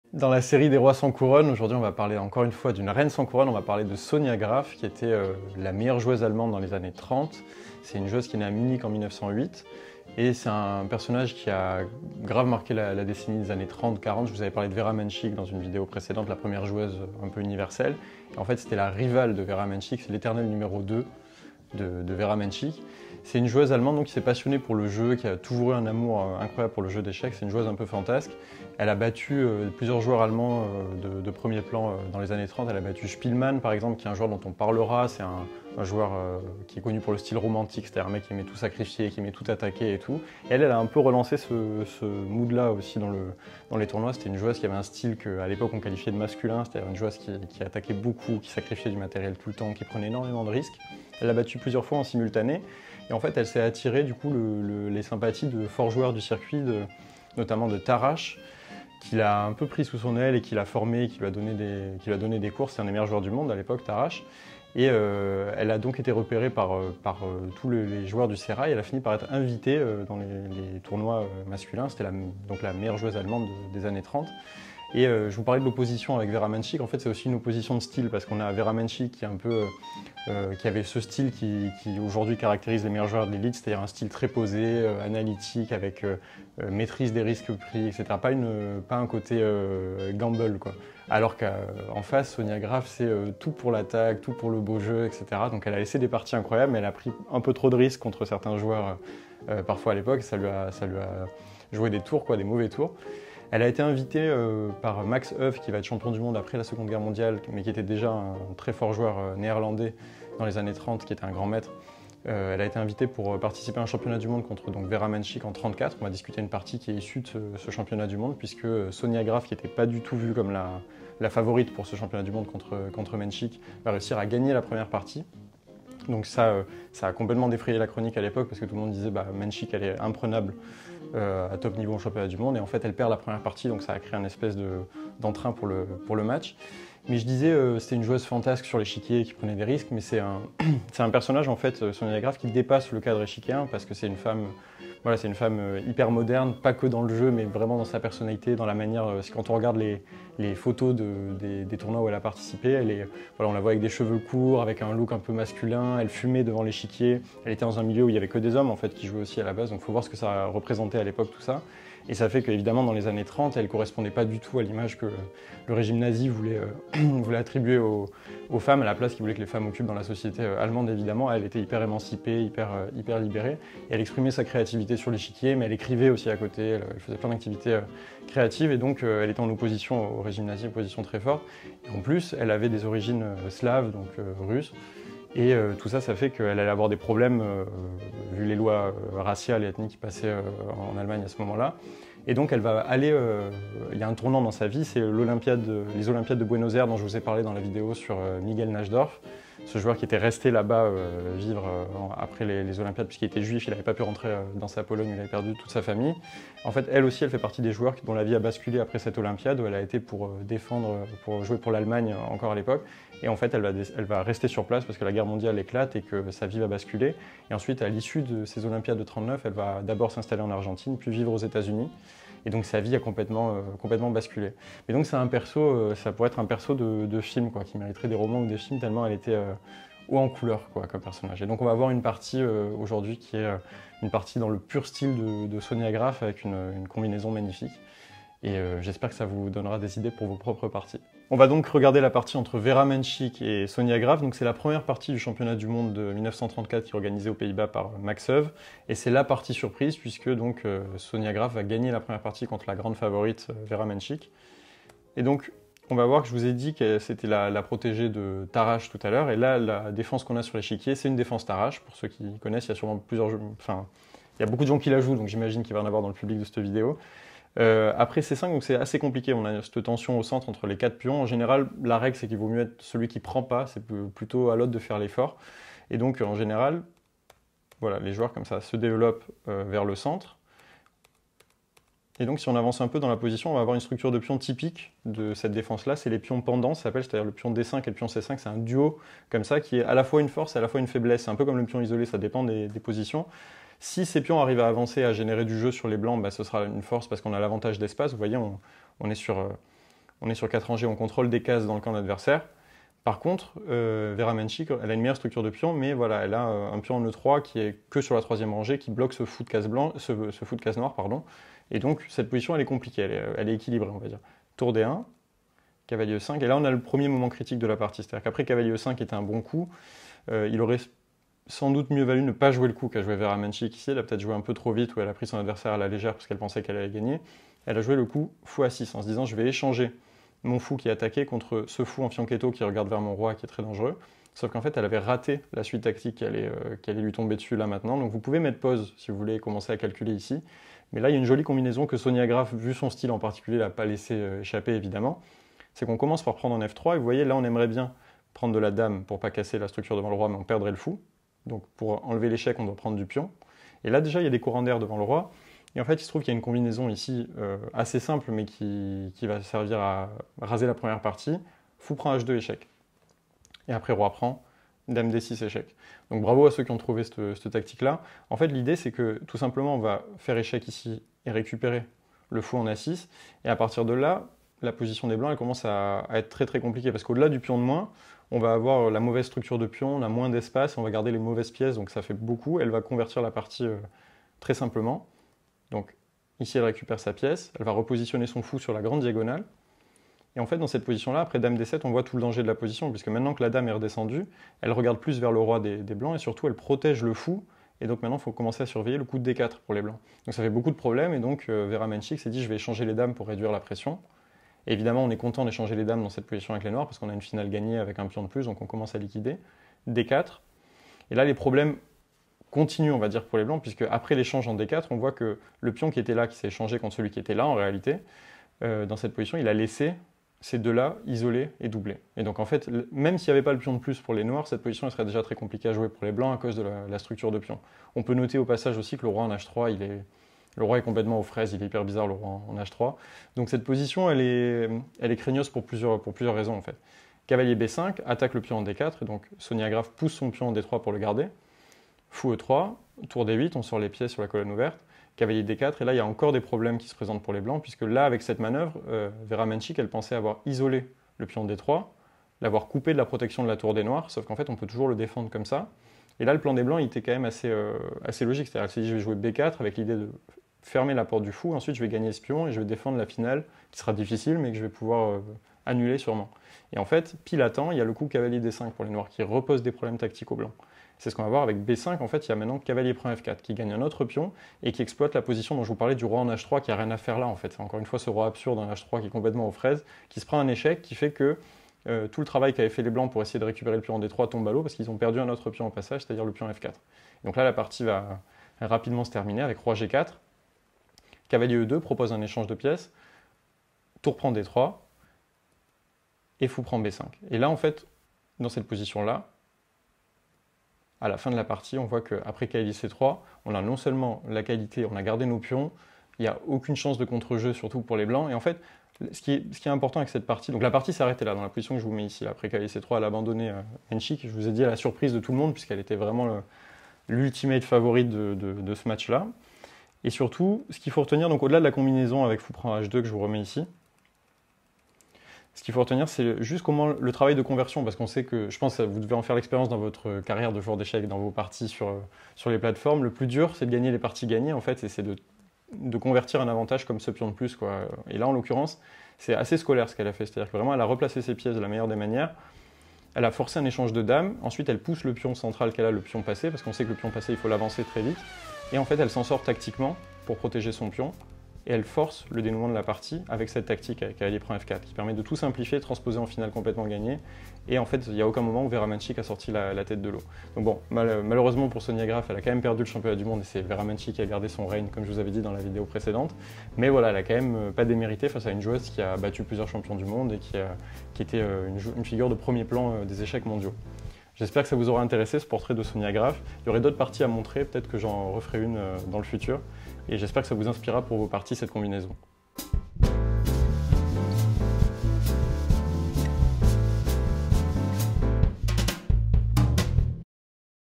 LA VRAIE BETH HARMON : SONJA GRAF, reine sans couronne
SONJA GRAF, une reine sans couronne

Aujourd'hui on parle d'une joueuse allemande du siècle dernier : Sonja Graf. Dans l'ombre de Vera Menchik, elle n'a selon moi pas bénéficié de la notoriété qu'elle mérite. Cette joueuse était bien en avance sur son temps et a affronté les meilleurs joueurs et joueuses de son époque. Sa carrière a néanmoins été affectée par le contexte international et la Seconde Guerre mondiale puisque elle a du fuir l'Allemagne et émigrer en Amérique...

On va ici parler d'une partie remportée contre sa rivale Vera Menchik avec les pièces noires.

Encore une vie digne d'un film,

Bon visionnage